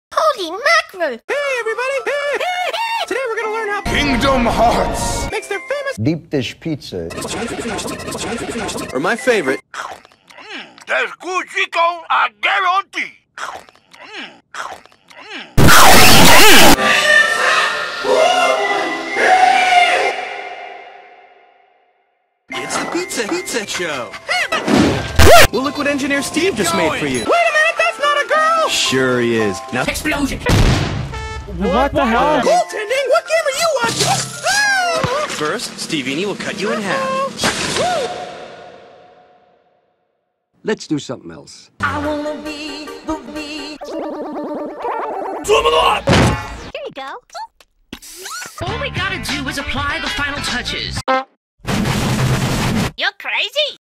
{YTP Tennis Round 1} Pizza Pizza Head Show with Inflated Jigglypuff
Round 1 of 5 against rer

rer is an underrated kind of YTPer that does make some good content as shown in his recent Pokemon YTP so I figure why not, I can make a Tennis video with him along with a few others I been getting. Thou he requested that I don't show any real life human faces for some reason.